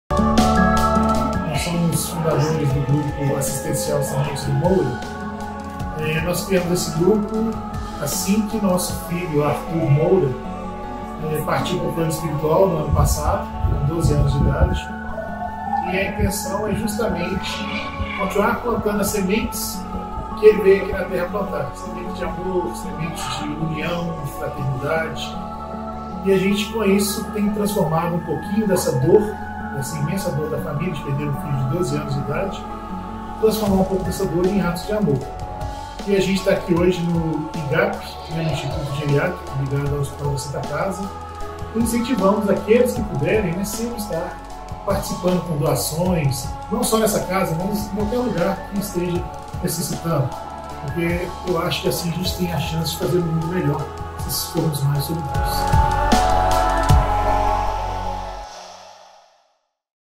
Solidariedade Doação de fraldas no IGAP
Os voluntários do Grupo Assistencial Sementes do Moura conseguiram arrecadar cerca de duas mil fraldas para os moradores do Instituto Geriátrico Afonso Pena (IGAP).

Se você também deseja fazer a sua doação, ligue para o número ou entregue pessoalmente na Provedoria da Santa Casa BH (Rua Álvares Maciel, 611, bairro Santa Efigênia, BH/MG). Ajudar faz bem para a saúde!